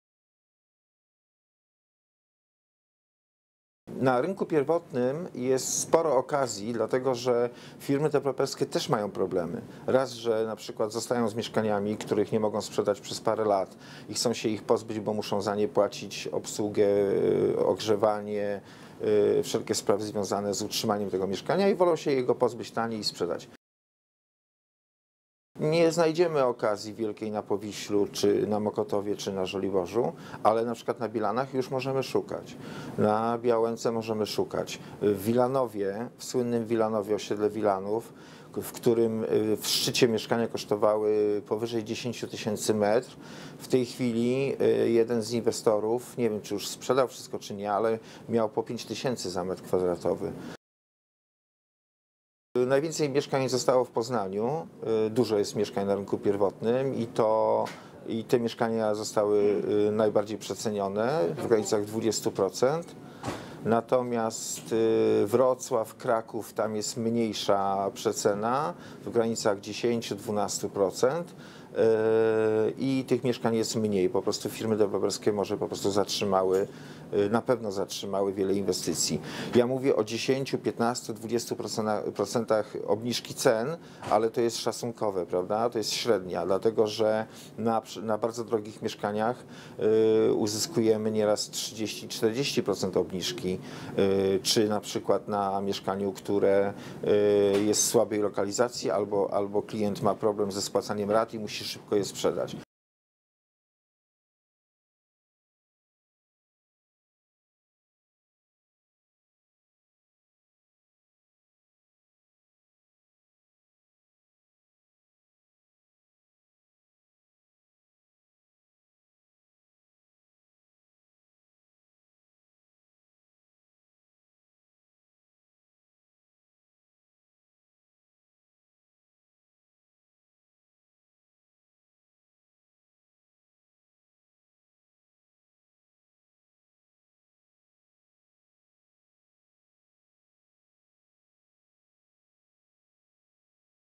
Ceny mieszkań na poziomie sprzed 10 lat
Na rynku nieruchomości trwa wyprzedaż. W ciągu ostatnich dwóch lat ceny mieszkań na rynku pierwotnym spadły średnio o kilkanaście procent - wynika z raportu agencji nieruchomości Emmerson. Bardzo drogie mieszkania tańsze są nawet o 1/3.

W ciągu dwóch ostatnich lat obniżki we Wrocławiu wyniosłu ponad 12 proc., w stolicy - 7,5 proc. Zdaniem eksperta z firmy Emmerson, na rynek wróciły ceny z lat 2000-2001.

 - Nie znajdziemy wielkiej okazji na Powiślu, Mokotowie czy na Żoliborzu, ale np. na Bielanach możemy szukać, na Białołęce też. W Wilanowie, na Osiedlu Wilanów, gdzie w szczycie mieszkania kosztowały 10 tys. zł za m kw., teraz jeden z inwestorów miał po 5 tys. zł za m kw. -- podkreśla Jarosław Skoczeń.

Badania firmy Emmerson wskazują, że w pięciu największych miastach Polski zwiększyła sie liczba gotowych mieszkań na rynku pierwotnym. W Warszawie średnio co czwarte nowe mieszkanie zostało już ukończone.

 - Najwięcej mieszkań zostało w Poznaniu. Dużo jest mieszkań na rynku pierwotnym i te mieszkania zostały najbardziej przecenione, w granicach 20 proc. Natomiast we Wrocławiu, w Krakowie jest mniejsza przecena, w granicach 10-12 proc. i tych mieszkań jest mniej. Może firmy deweloperskie wstrzymały wiele inwestycji - ocenia ekspert.

Ponieważ deweloperzy zwykle starają się sprzedać jak najwięcej lokali przed ukończeniem inwestycji, duża liczba gotowych mieszkań może świadczyć o problemach firm ze znalezieniem nabywców.

Mniejszy popyt niż podaż na rynku nieruchomości daje kupującym korzystną pozycję negocjacyjną.

 - Ja mówię o 10-15 proc. obniżki cen, ale to jest średnia. Na bardzo drogich mieszkaniach uzyskujemy nieraz 30-40 proc. obniżki. Albo na mieszkaniach w słabej lokalizacji czy kiedy właściciel ma problem ze spłacaniem rat i musi je szybko sprzedać - podkreśla Skoczeń.

W dowód na to przytacza szczegóły oferty w podwarszawskich Ząbkach, gdzie dom o powierzchni ponad 100 m kw. z 400-metrową działką sprzedawano za niecałe 300 tys. zł.

Na największe promocje możemy natrafić przy inwestycjach, które są w początkowej fazie realizacji. Pieniądze ze sprzedaży pierwszych lokali na wstępnym etapie prac pozwalają deweloperom na realizację kolejnych. Zwłaszcza, że banki przed udzieleniem kredytu na nowy projekt oczekują, że część z nieistniejących jeszcze lokali będzie miała już swoich nabywców. Dlatego firmy są skłonni do obniżek w czasie negocjacji.

Warto też sprawdzić już ukończone inwestycje.

 - Firmy deweloperskie zostają z mieszkaniami, których nie mogą sprzedać przez parę lat i chcą się ich pozbyć, bo muszą za nie płacić - za obsługę, ogrzewanie, wszelkie koszty związane z utrzymaniem tego mieszkania, więc chcą się go pozbyć i sprzedać -- mówi Jarosław Skoczeń z agencji nieruchomości Emmerson.

Klienci muszą się liczyć z tym, że ostatnie niesprzedane mieszkania mogą być mniej atrakcyjne, na przykład przez gorszy rozkład czy widok z okna.